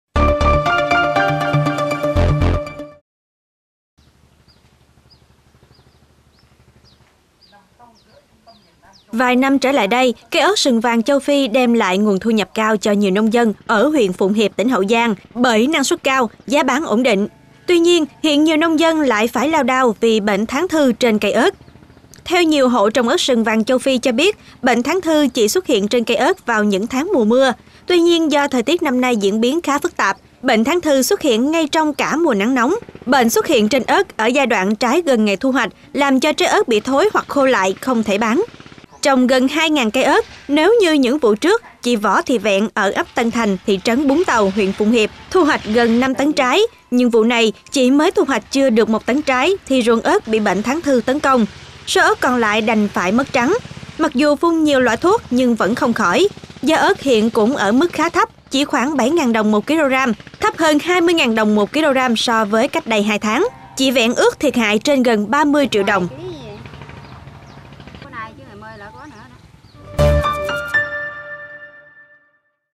Nông dân trồng ớt lao đao vì bệnh thán thư
Vài năm trở lại đây, cây ớt sừng vàng châu Phi đem lại nguồn thu nhập cao cho nhiều nông dân ở huyện Phụng Hiệp, tỉnh Hậu Giang, bởi năng suất cao, giá bán ổn định. Tuy nhiên, hiện nhiều nông dân lại phải lao đao vì  bệnh thán thư trên cây ớt.